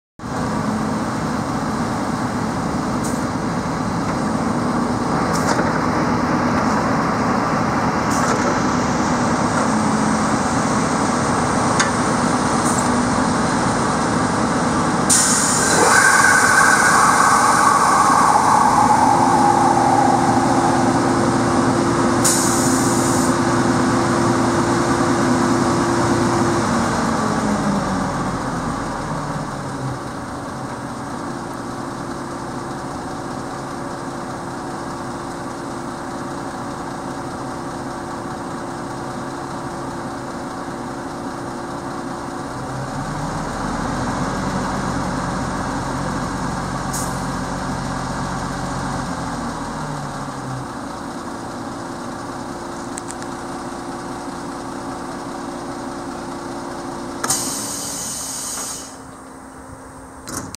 MAN TGS 26.440 LL - 10 905403
MAN TGS 26.440 LL - 10, Göteborg/Angered, Auktionsdatum: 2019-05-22, 279112 km, Helhetsintrycket är gott.

MAN TGS 26,440 LL 6x2 Slam vacuum truck - 10, Göteborg/Angered, AuctionDate: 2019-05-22, 279112 km, The overall impression is good.